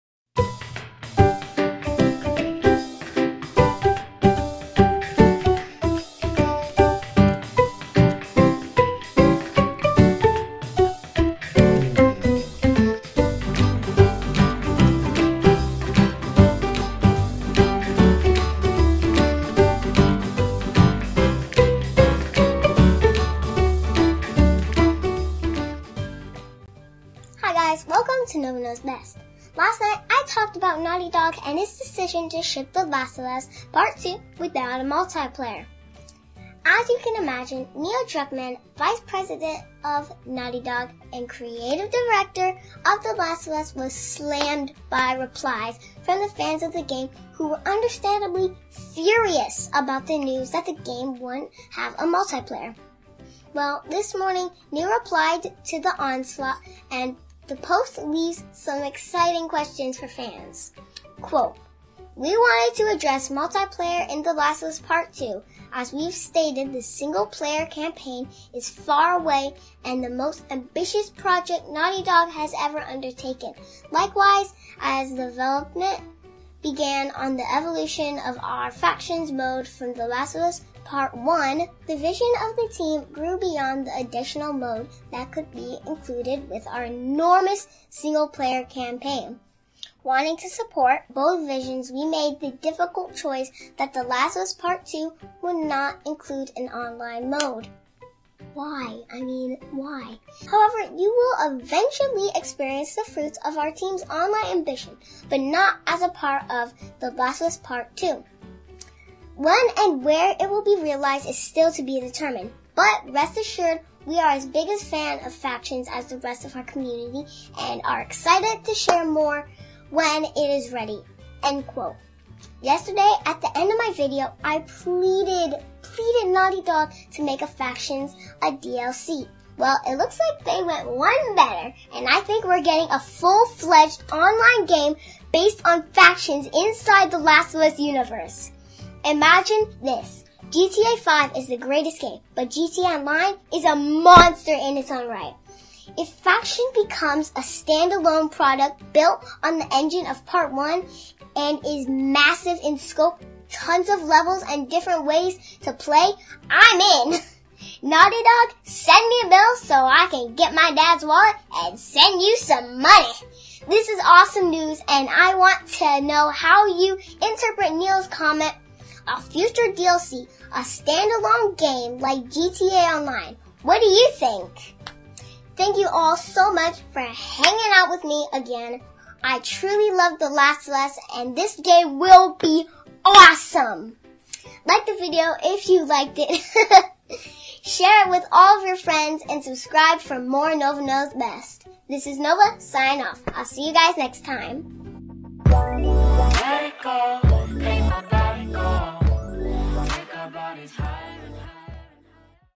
the Last Of us part 2 Will Have Multiplayer!! Factions PS5 PS% factions
What do you think? A DLC, A standalone or something else???
Let me know and let me know what you'd like to see me talk about next! Nova, signing out!

Please visit the Facebook community

PSN:
thebeastlygamer-

Xbox Live:
Iambeastlygamer

Laurent Caccia